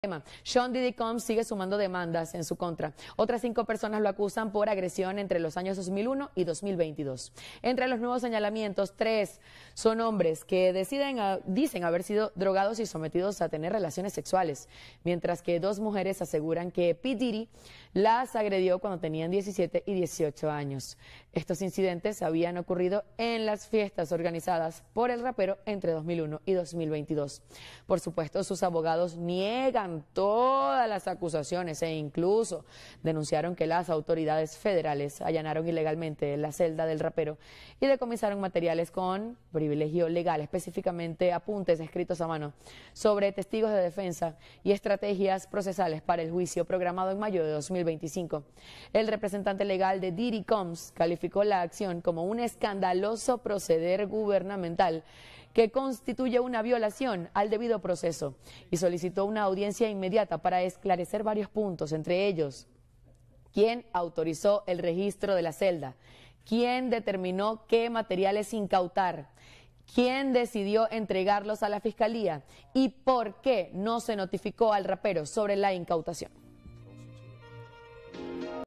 Otras cinco personas acusaron a Sean “Diddy” Combs de agresión - 20 de noviembre de 2024
Sean “Diddy” Combs sigue sumando demandas en su contra. Otras cinco personas lo acusan por agresión entre los años 2001 y 2022.